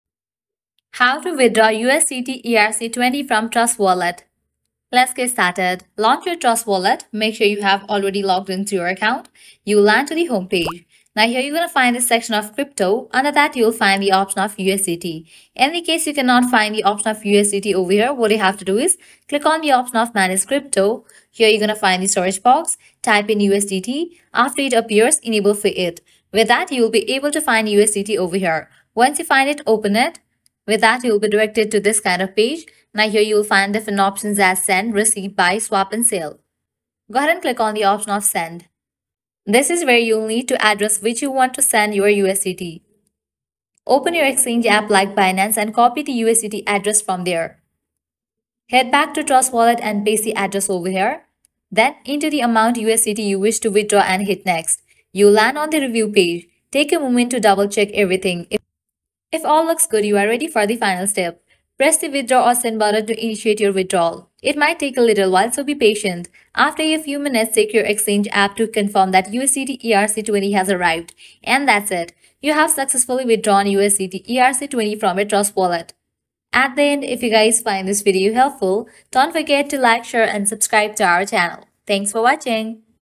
How to Withdraw USDT ERC20 from Trust Wallet (Full Guide)
Looking to withdraw USDT ERC20 from your Trust Wallet in 2024? This easy guide provides step-by-step instructions on how to transfer your USDT ERC20 tokens from Trust Wallet to another wallet or exchange. Follow these straightforward steps to complete your withdrawal securely.

In this tutorial:
0:00 - Introduction
0:15 - How to Withdraw USDT ERC20 from Trust Wallet in 2024 (EASY)

Learn how to effortlessly withdraw USDT ERC20 from your Trust Wallet in 2024 with this simple guide. This video covers the entire process, from accessing your wallet to confirming the transaction, ensuring your USDT ERC20 tokens are transferred securely.

Manage your crypto transactions effectively by following this guide to withdraw USDT ERC20 from Trust Wallet!